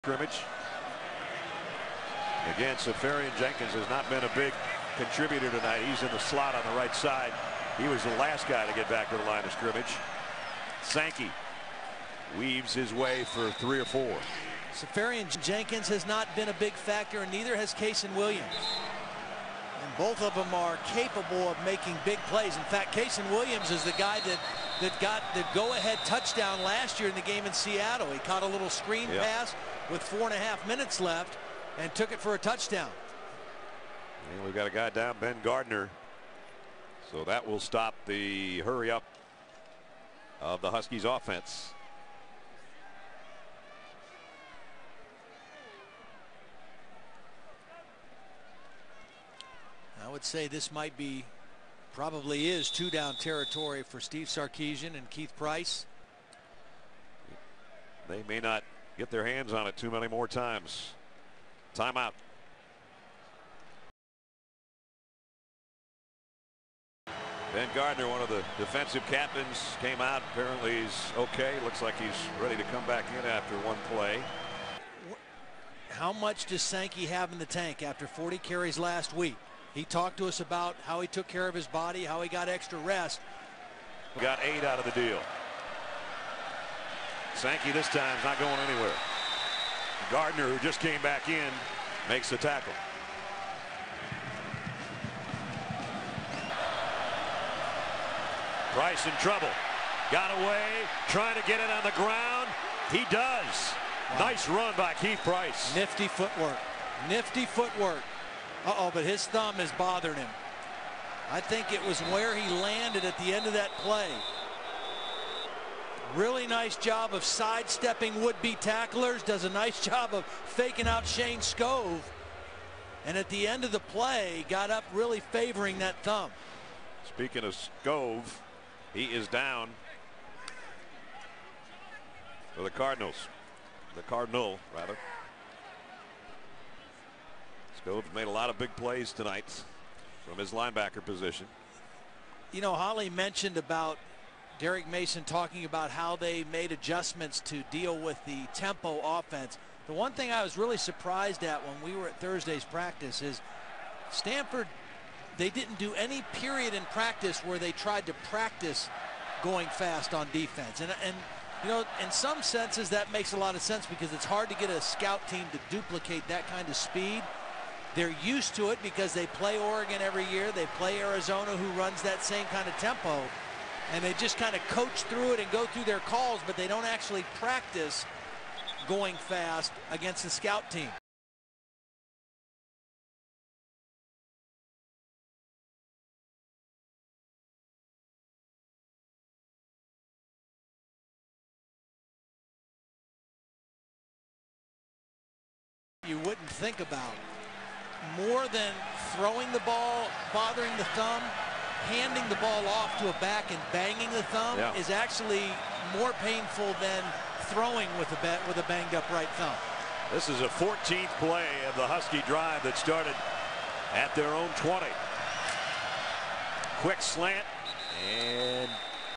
Stanford Faking Injuries?
The Stanford defense has two injuries against the fast-paced Washington offense, which slowed down the tempo and gave the Cardinal defense a chance to rest. Ben Gardner re-entered the game after one play (as the rule mandates) while Shayne Skov ran back onto the field after two plays.